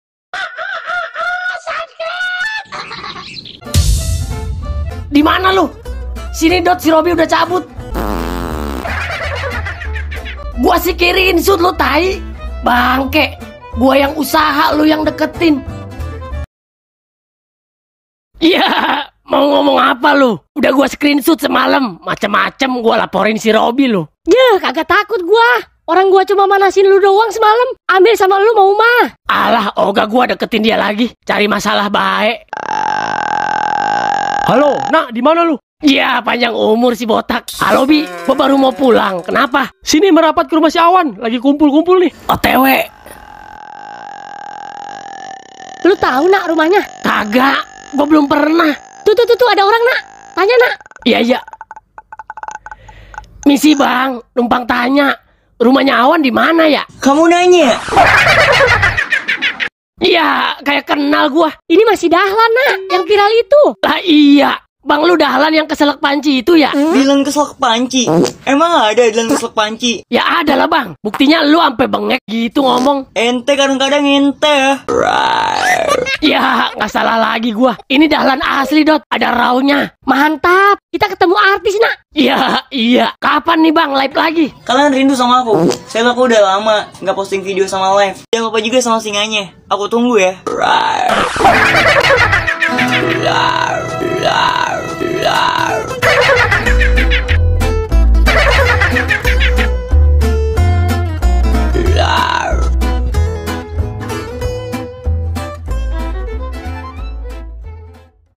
KAMU NANYA? Animasi Sekolah
Animasi yang menceritakan masa masa sekolah SMA, yang seru, lucu, kocak dan menghibur.